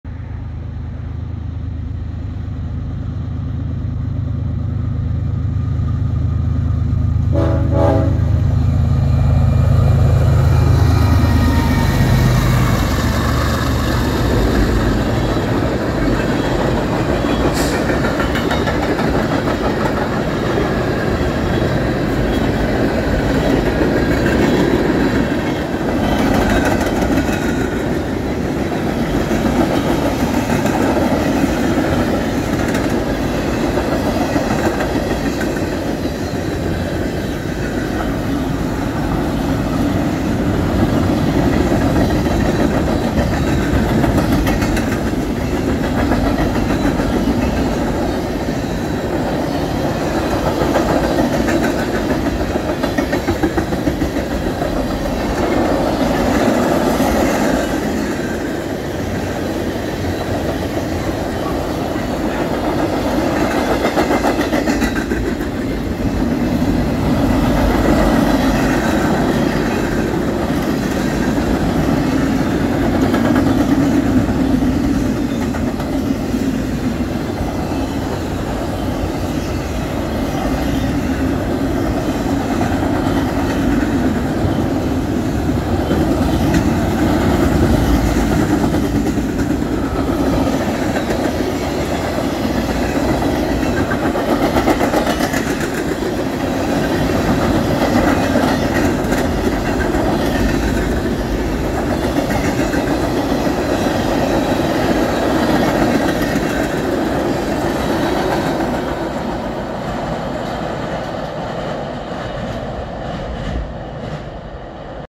CN M33491 with BNSF power on the Bluford Sub!!!
I caught CN M33491 (Kirk Yard-Gary, IN to Harrison Yard-Memphis, TN) after a short day at work. I wanted to shoot it at Rust Junction near Akin, IL, but the thunderstorm that blew in made me go farther south to Ferber.